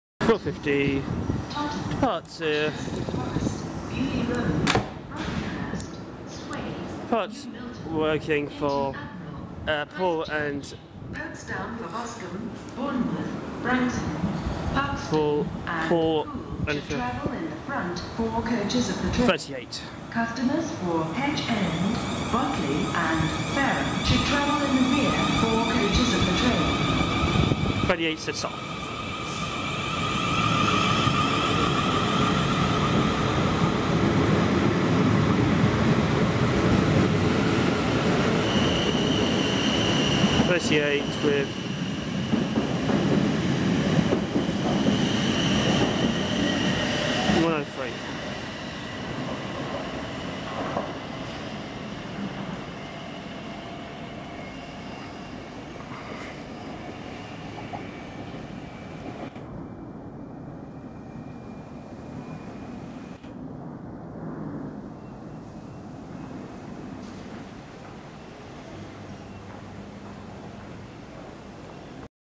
South Western Railway Class 450 038-450 103 Departure Woking for Poole and Fareham
South Western Railway Class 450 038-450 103 to Poole and Fareham Calling at Woking,Basingstoke,Micheldever,Winchester,Shawford and Eastleigh where the train divide the front four car continuing to Poole Calling at Southampton Airport Parkway,Southampton Central,Totton,Ashurst (New Forest),Beaulieu Road,
Brockenhurst,Sway,New Milton,Hinton Admiral,Christchurch,Pokesdown for Boscombe,Bournemouth,Branksome,Parkstone and Poole. And rear four car will go to Fareham Calling at Hedge End,Botley and Fareham.